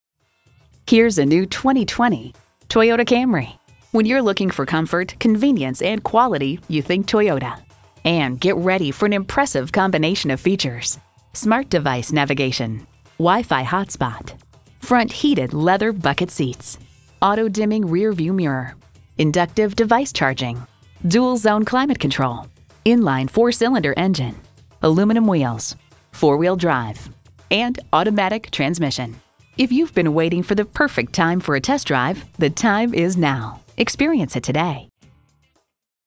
New 2020 Toyota Camry Rochester, MN S21400 - SOLD
Year: 2020
Make: Toyota
Model: Camry
Trim: XSE
Engine: 2.5L I4 DOHC 16V
Transmission: 8-Speed Automatic
Color: Blue
Interior: Ash Perforated Lth
Stock #: S21400
VIN: 4T1K61BK0LU018101
2020 Toyota Camry 4D Sedan 2.5L I4 DOHC 16V AWD XSE Blue Recent Arrival! 25/34 City/Highway MPGbrbrEvery new vehicle comes with a Lifetime Powertrain Warranty! See dealer for details. We look forward to earning your business.

Address:
4365 Canal Place Southeast